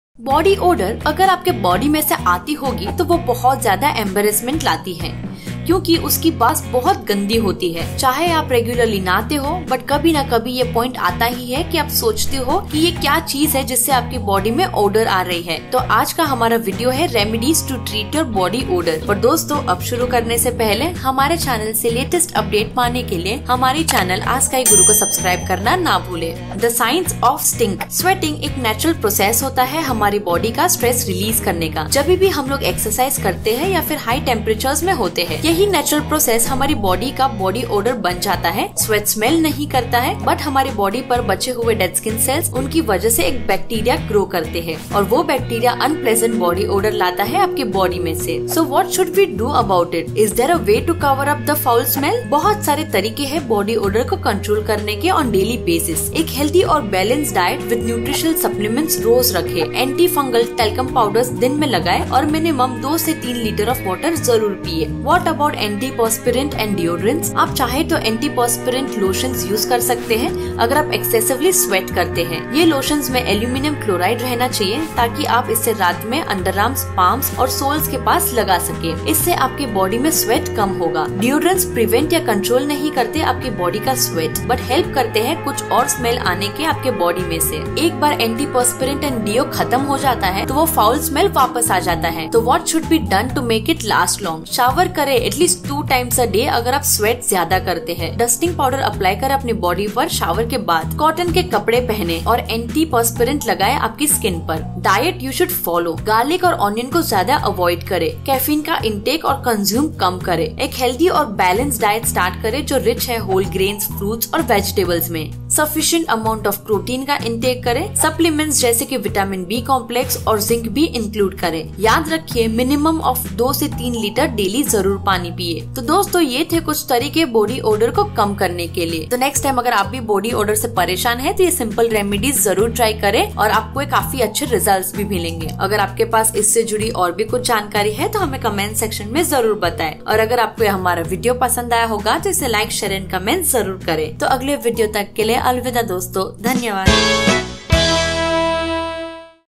HOW TO REMOVE SWEAT SMELL FROM BODY AND BODY ODOR PERMANENTLY- NATURAL WAYS TO GET RID OF BODY ODOR
HOW TO REMOVE SWEAT SMELL FROM BODY AND BODY ODOR PERMANENTLY- NATURAL WAYS TO GET RID OF BODY ODOR - ASKIGURU

In this video we have covered few tips on how to remove sweat smell from body and body odour permanently in a natural way.
1. The Science of Stink
2. So what should we do about it? Is there a way to cover up the foul smell?
3. What about antiperspirant and deodorants?
4. Once my antiperspirant and deo wears off, the foul smell comes back. What should be done to make it last long
5. Diet you should follow